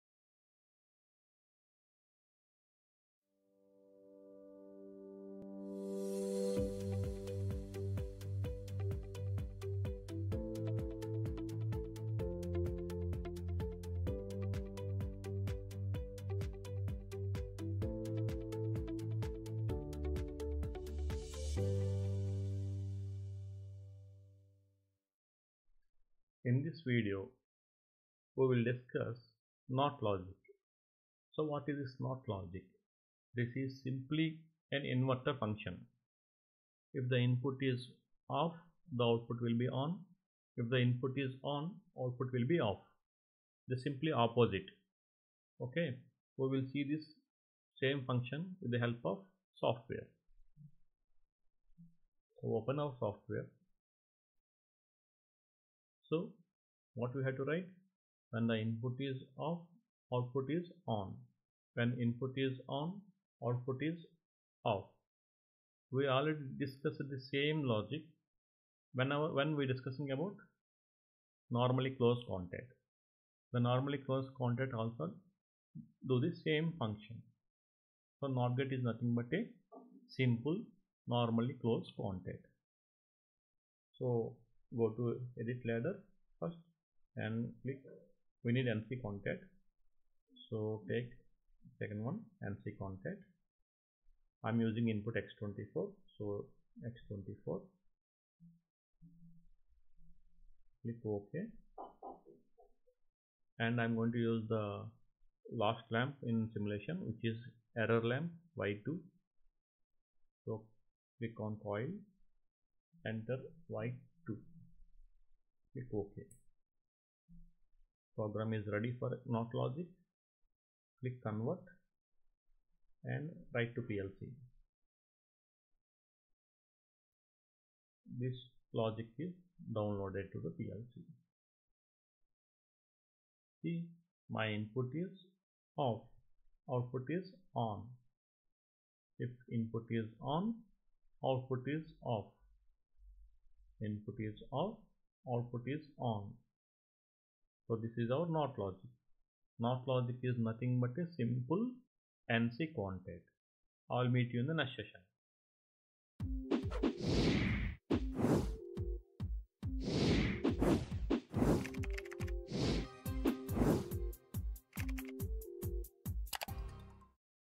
PLC NOT Logic - PLC Logic Functions
In this video, we learn how to create a Not Logic Gate in PLC ladder logic programming.

Not logic is also called an inverter function.